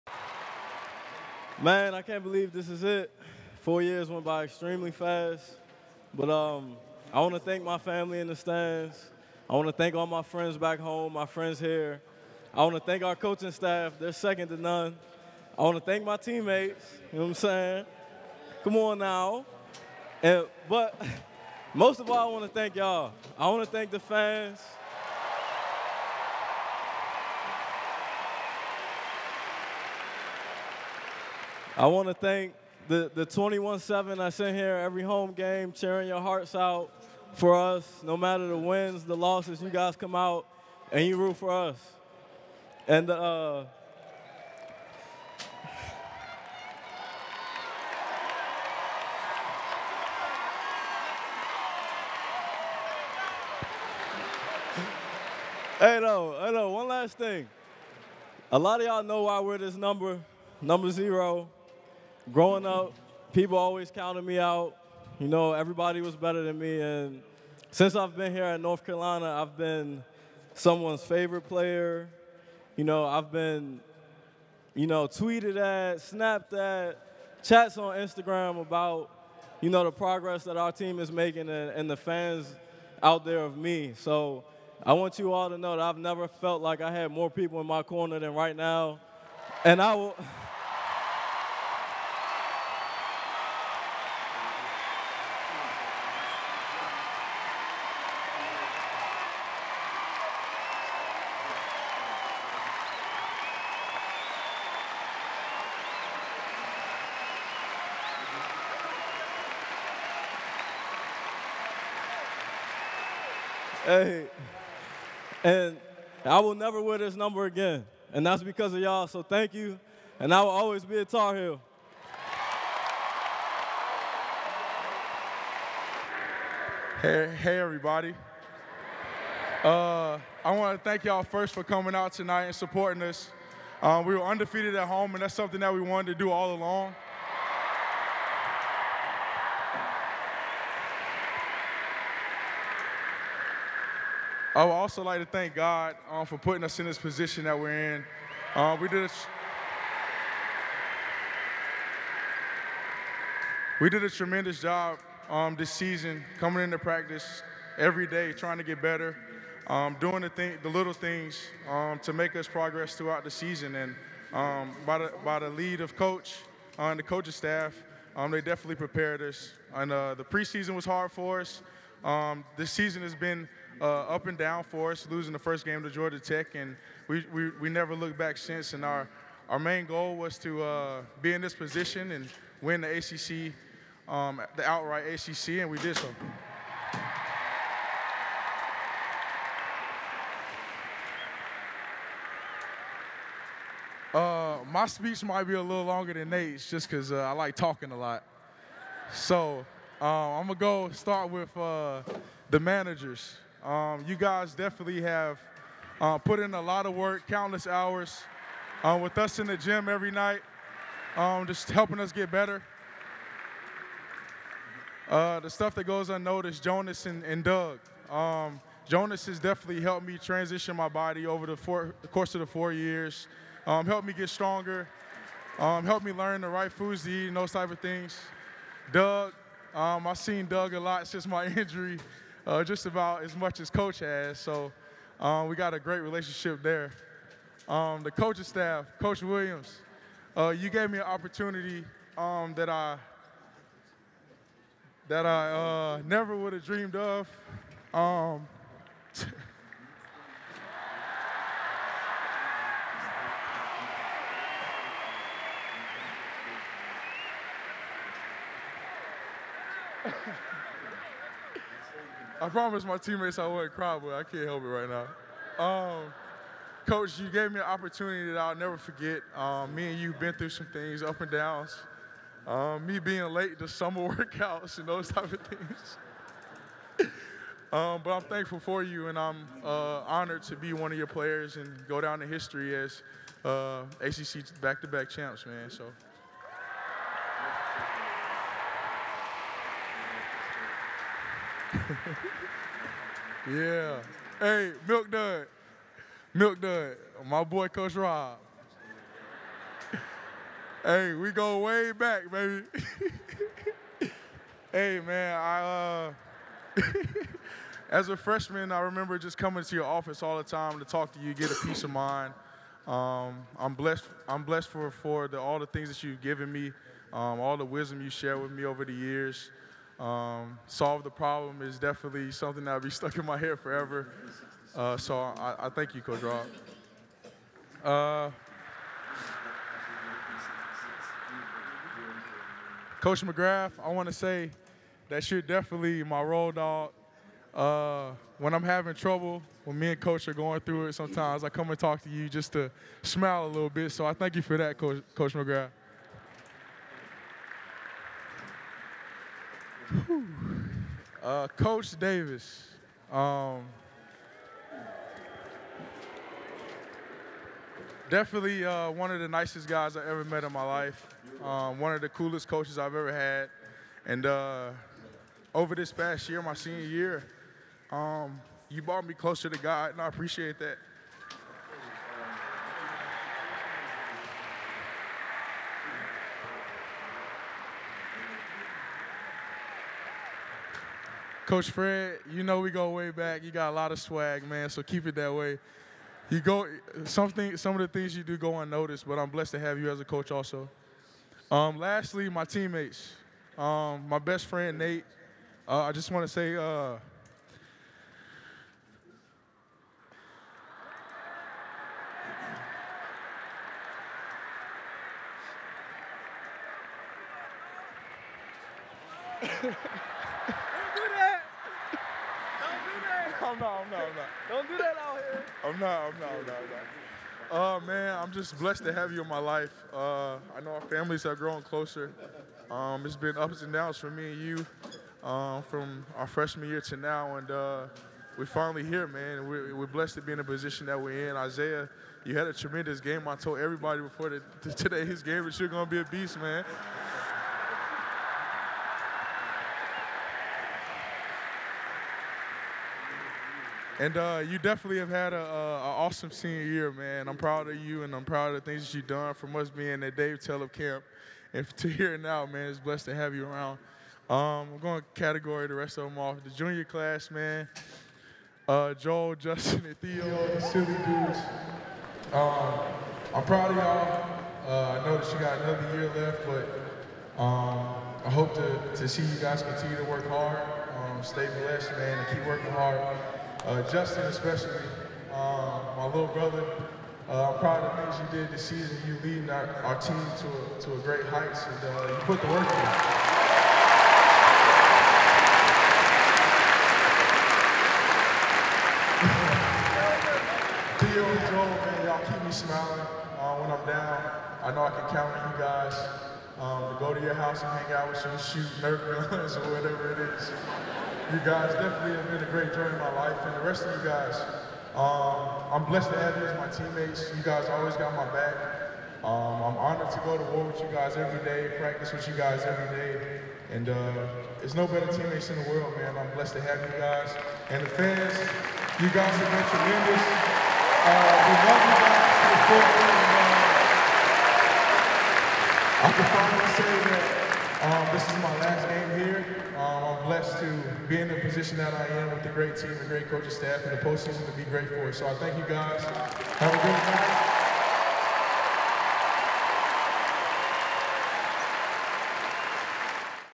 UNC Men's Basketball: Britt & Meeks Give Senior Speeches
Seniors Nate Britt and Kennedy Meeks spoke to the Smith Center crowd following the team's win over Duke on March 4, 2017.